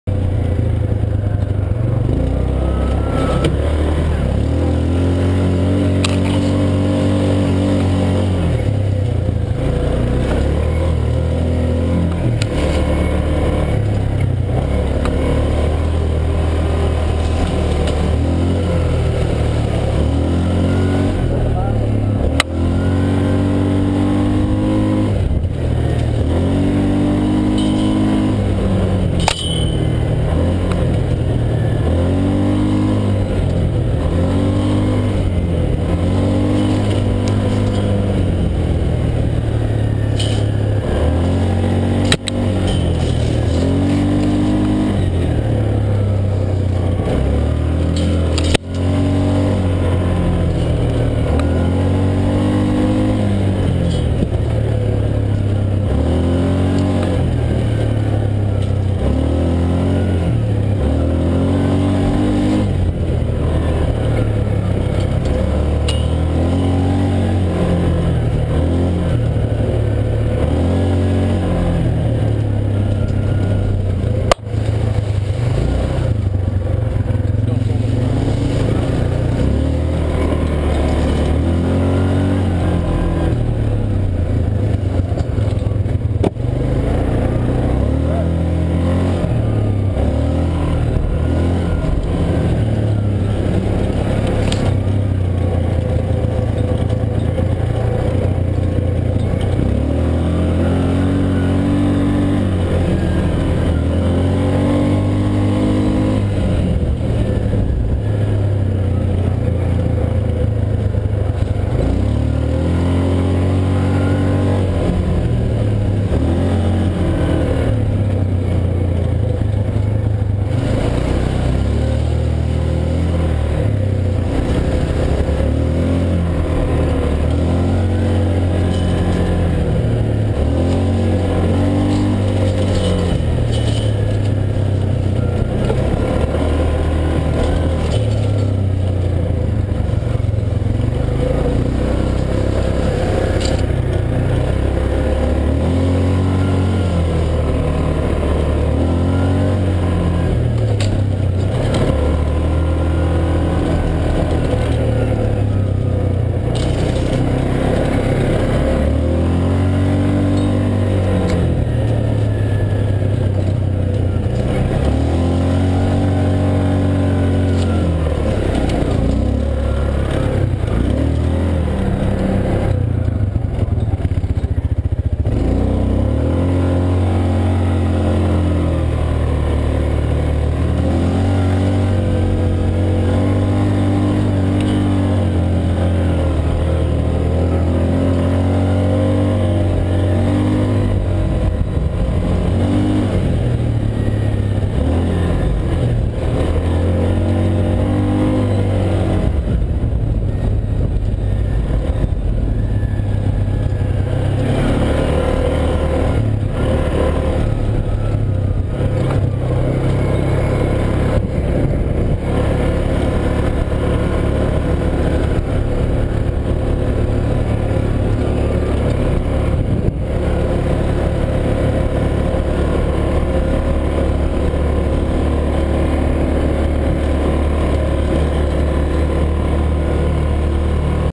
out on the Rhino
Just heading out on the Rhino with our neighbors.  When you live in the sticks, this is what you do!  Best part is this is a 1/4 mile from our house.